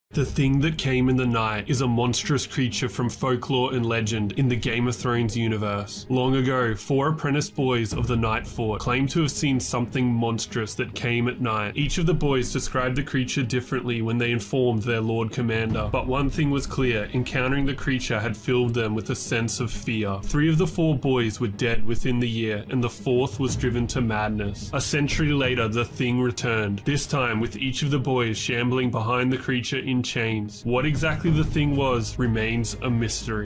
The Thing That Came In The Night Explained Game of Thrones House of the Dragon ASOIAF Lore
The Thing That Came In The Night Explained A song of ice and fire/Game of Thrones/ House of the dragon

Backing Track
BONNIE GRACE